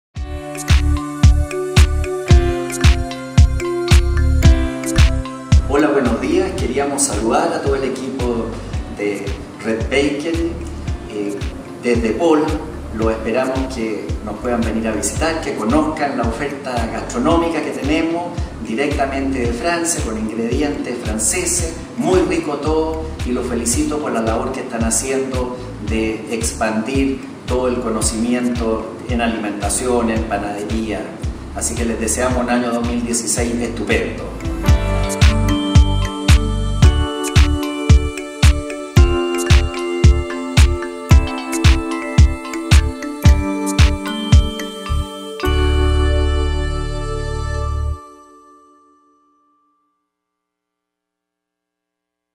PAUL Boulangerie Chile saluda a RedBakery
Con motivo de una reciente entrevista que realizamos a Max Winter, representante de Paul Boulangerie en Chile, dejamos registrado en video su saludo con deseos de un exitoso 2016.